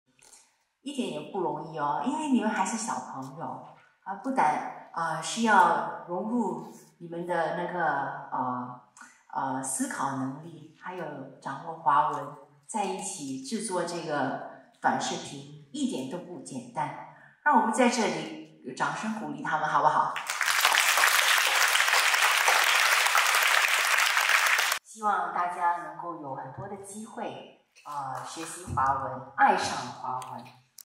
Singapore Centre for Chinese Language National Vlog Competition 2023 l Video Production in Singapore
Firefly Photography presents Singapore Centre for Chinese Language National Vlog Competition 2023

About:
Join the journey of language and culture at the Singapore Centre for Chinese Language National Vlog Competition 2023. Our highlight video captures the essence of this exciting event, where participants emphasize the significance of the Chinese language through engaging vlogs. Explore the vibrant tapestry of Chinese culture and language, as we highlight the importance of this rich heritage.